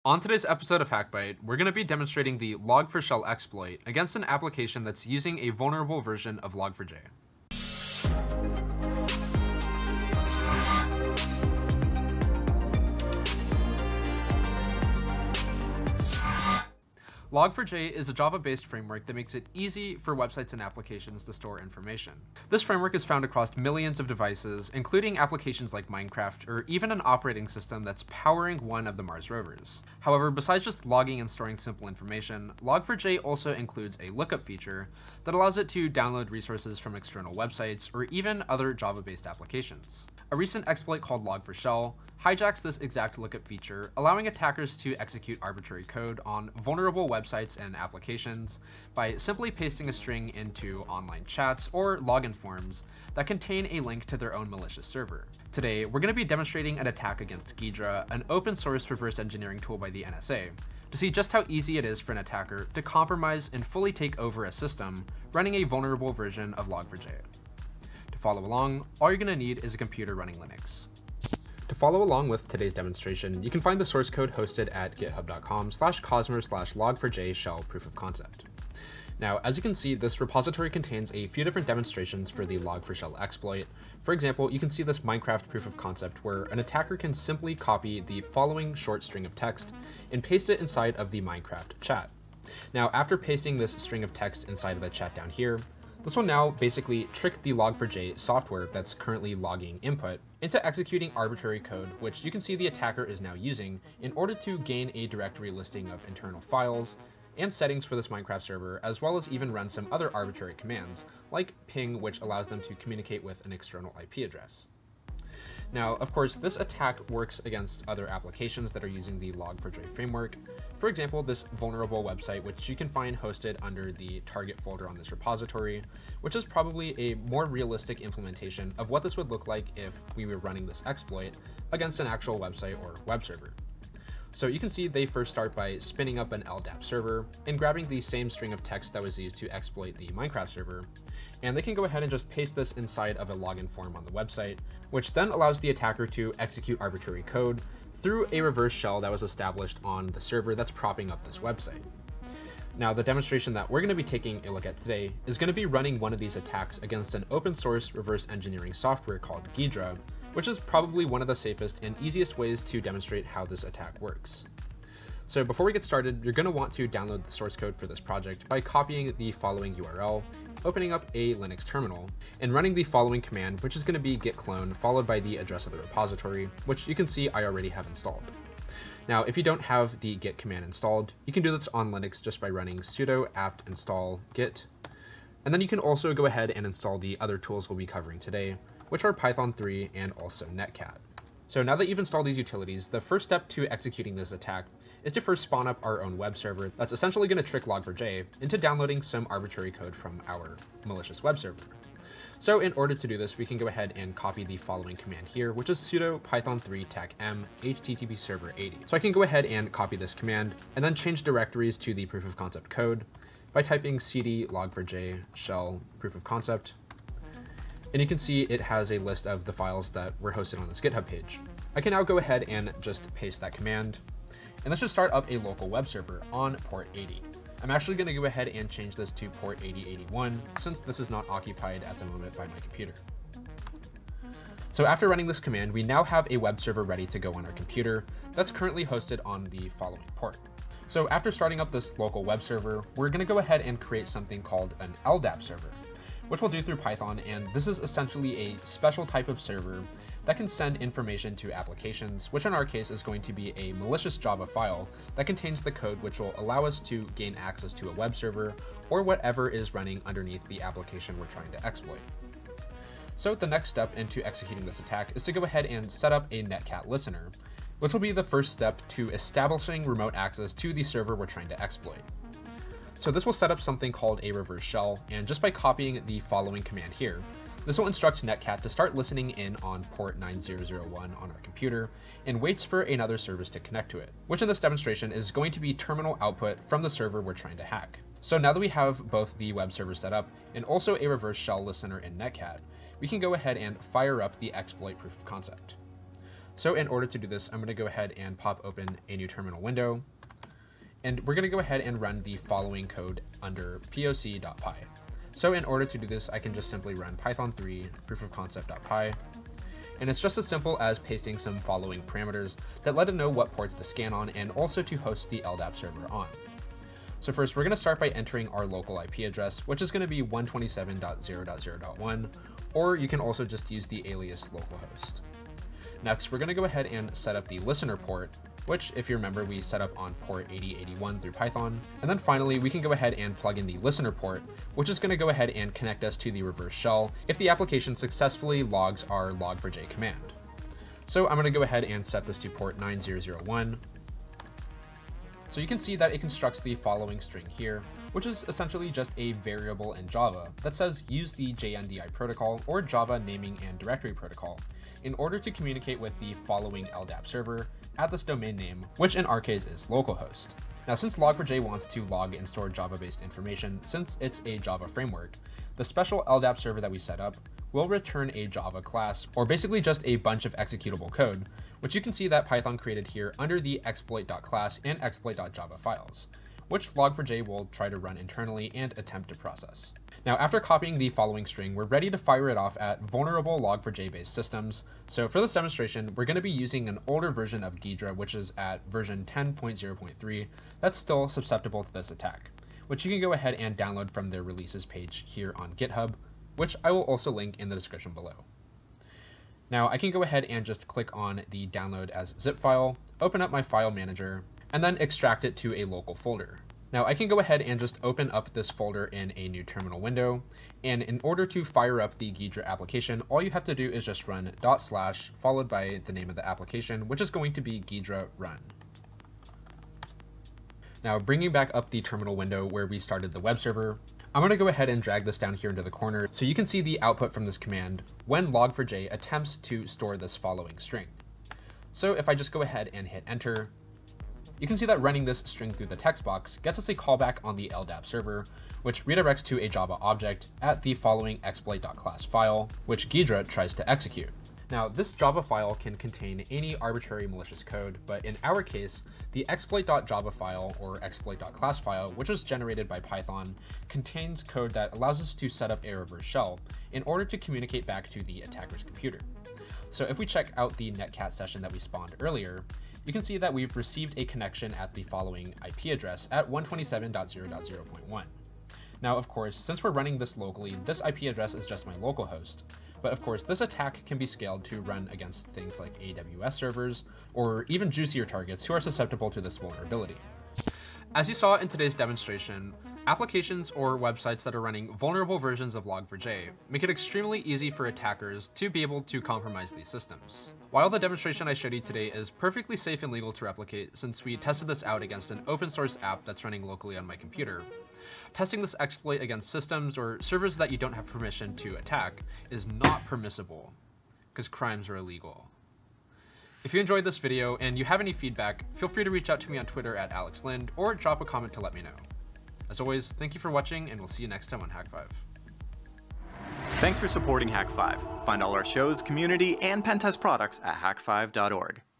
How Hackers Exploit Log4J to Get a Reverse Shell (Ghidra Log4Shell Demo) | HakByte
On this episode of HakByte, @AlexLynd  demonstrates a Log4Shell attack against Ghidra, and shows how a reverse shell can be established on compromised systems running the vulnerable Log4J Java framework.
This framework runs on millions of Java powered devices and was recently exploited, exposing a dangerous vulnerability that uses a single line of code to hack vulnerable systems.

Chapters:
Intro @AlexLynd  00:00
What is Log4J? 00:16
Log4Shell Exploit Explained 00:40
Vulnerable Programs 01:11
Set up the Log4Shell Demo 02:33
Create a Webserver 03:11
Netcat Reverse Shell Listener 04:01
Set up Log4Shell Demo 05:01
Log4Shell String Explained 05:45
Ghidra Setup 06:24
Log4Shell Attack Demo 07:01
Netcat Reverse Shell 07:39
Outro 08:00